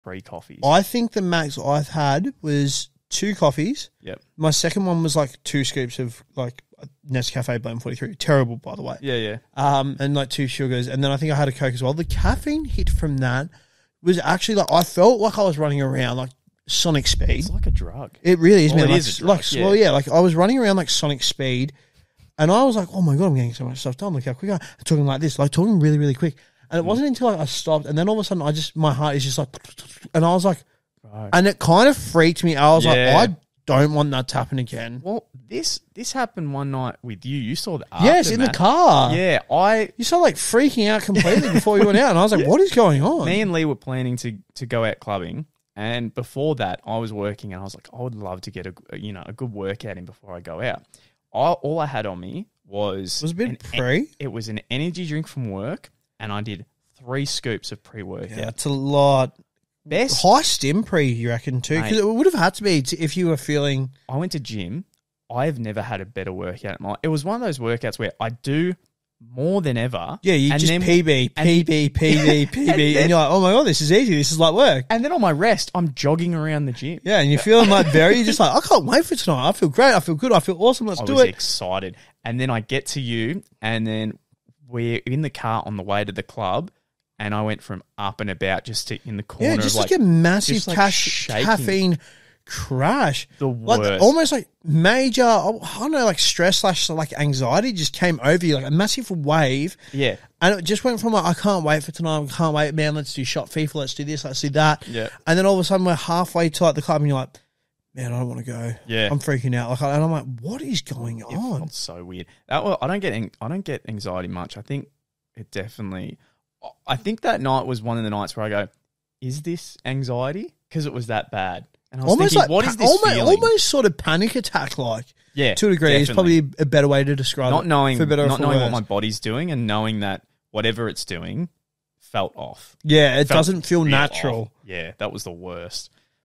Has This Happened To You?!
In this clip we talk about caffeine and how it effects us and also the time Luke had a panic attack!